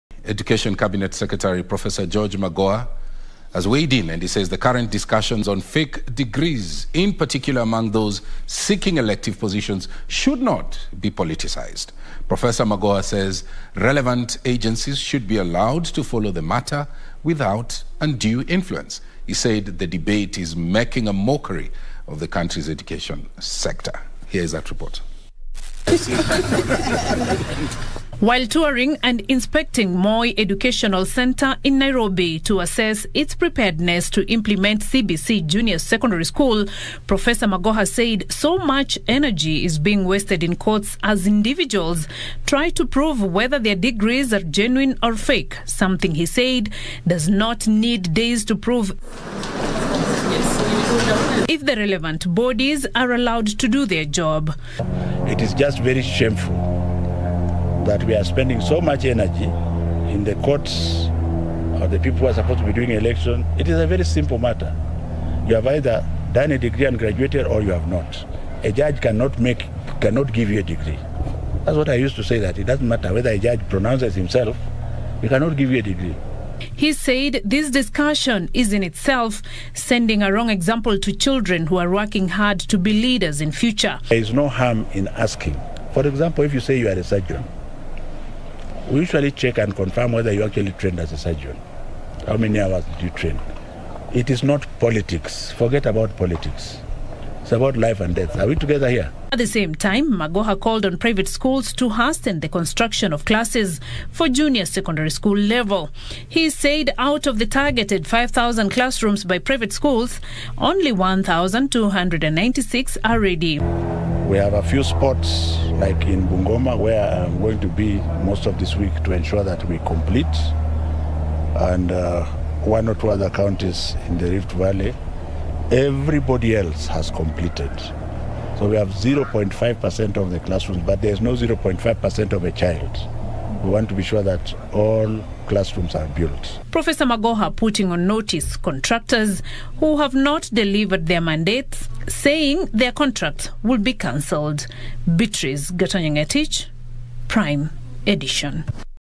Magoha ; "Current discussions on fake degrees should not be politicized."
Education Cabinet Secretary Prof. George Magoha says the current discussions on fake degrees especially for those seeking elective posts should not be politicized. Magoha said relevant agencies should be allowed to follow on the issue  without being influenced and put to task those being questioned.